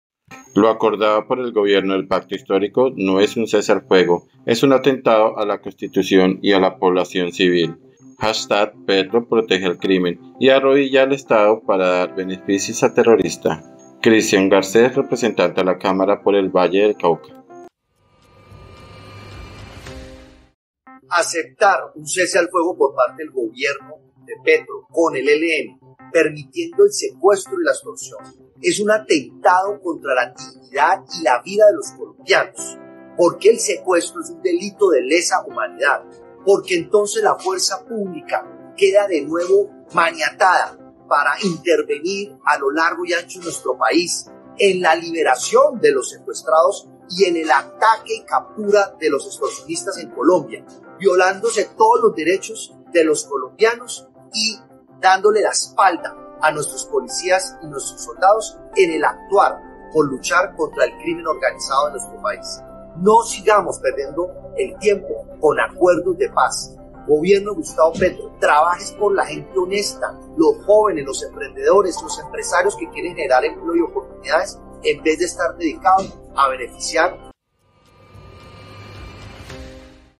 🔴 DENUNCIA CHRISTIAN GARCES LO ACORDADO POR EL GOBIERNO DEL PACTO HISTÓRICO
Lo acordado por el Gobierno del pactohistórico no es un cese al fuego, es un atentado a la Constitución y a la población civil. PetroProtegeAlCrimen y arrodilla el Estado para dar beneficios a terroristas.
Christian Garcés